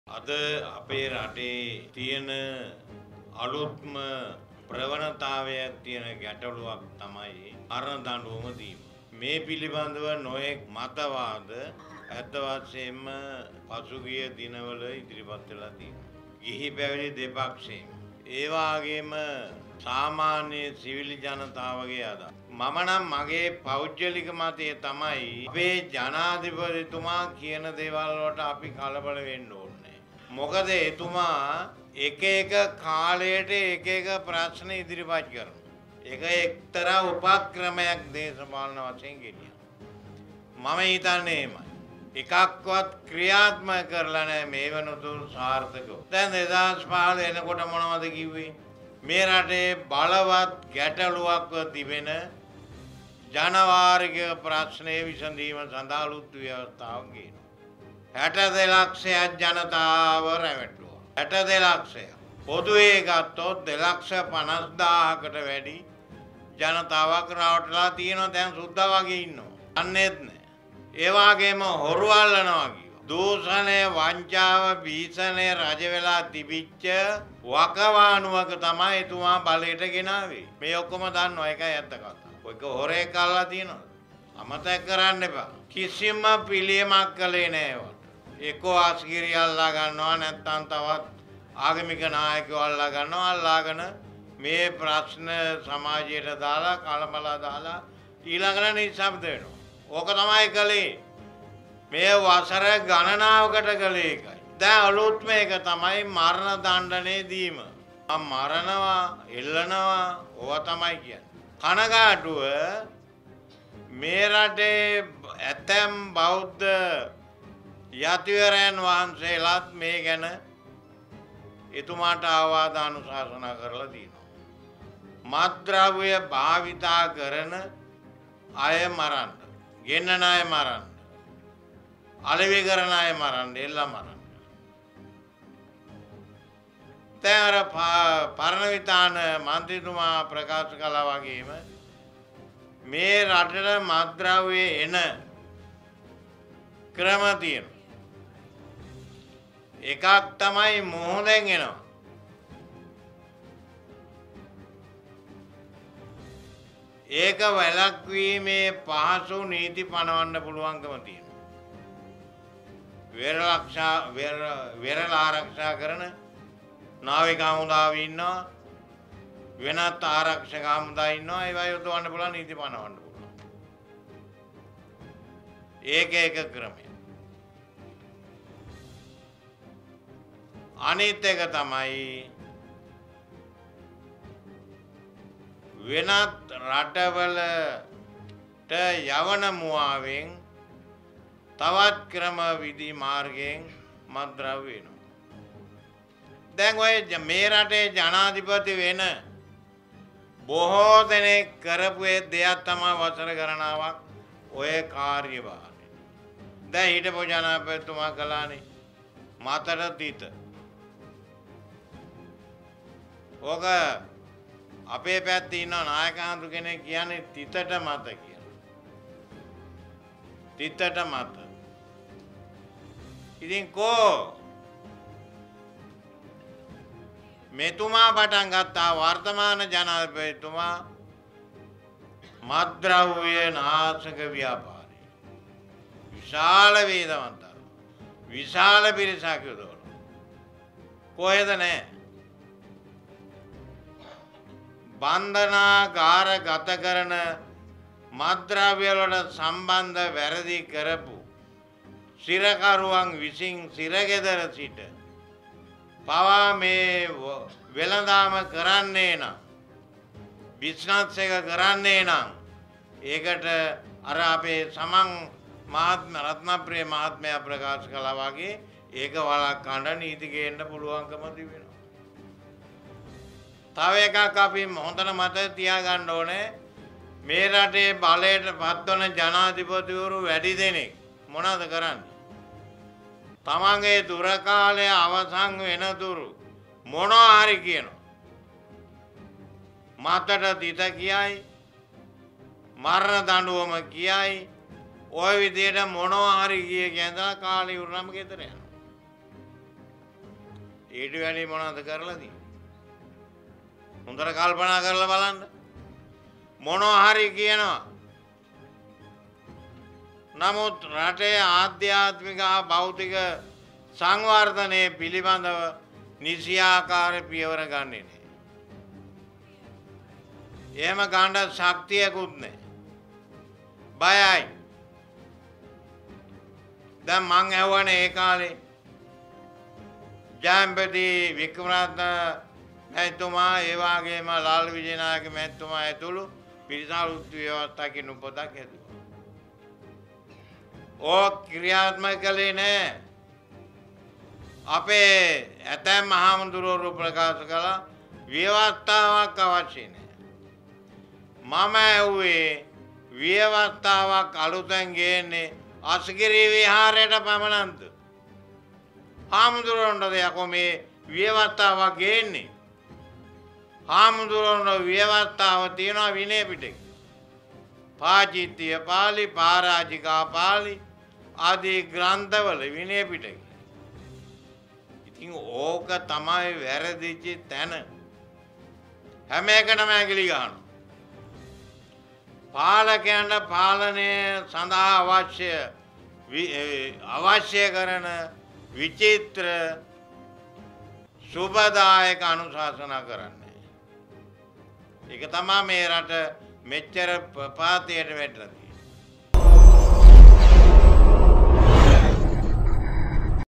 ජනාධිපතිතුමා දෙලක්ෂ පනස් දහසක ජනතාවක් රවට්ටලා දැන් සුද්දා වගේ ඉන්නවා | Breaking.lk
ජනාධිපතිතුමා දෙලක්ෂ පනස් දහසක ජනතාවක් රවට්ටලා දැන් සුද්දා වගේ ඉන්නවා